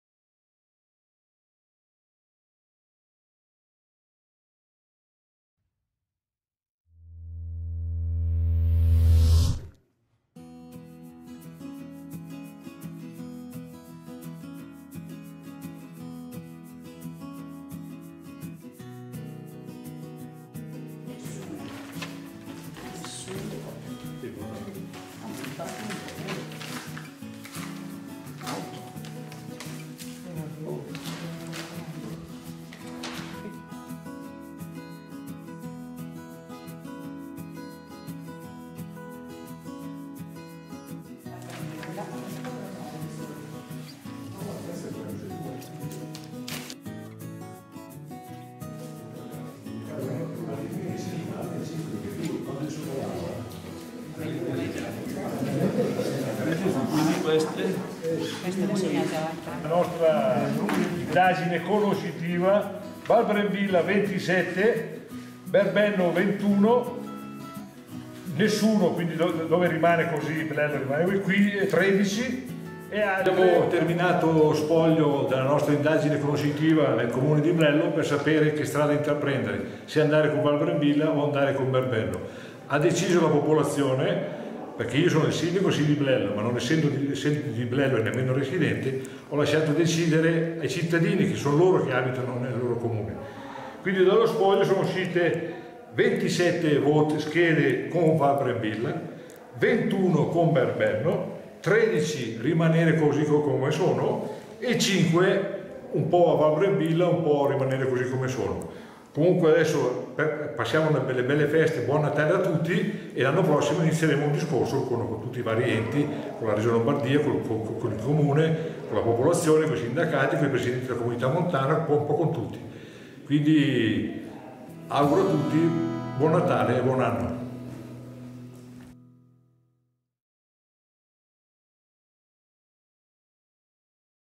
Blello va verso la fusione con Val Brembilla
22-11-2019, Blello - Successo per la consultazione informale: votano in 66 su 75. In testa Val Brembilla. Due abitanti su 3 non vogliono più restare da soli. Val Brembilla ha raccolto 27 voti, Berbenno 21. Solo in 13 vogliono restare così. Ora faremo tutti i passi necessari, spiega il sindaco.